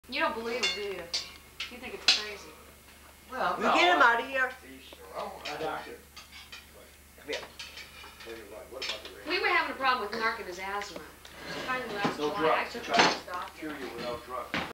Mark hates the camera
When Jeff doesn't stop filming, Mark gets MAD.